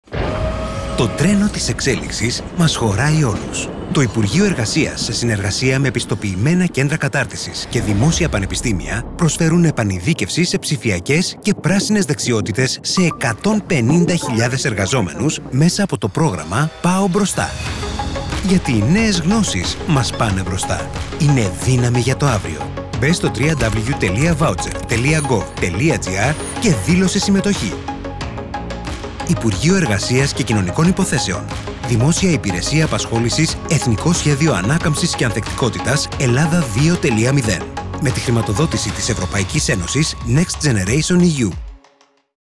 Πρόγραμμα «Πάω Μπροστά»
Το πρόγραμμα του υπουργείου Εργασίας αφορά την αναβάθμιση δεξιοτήτων για 150.000 εργαζόμενους.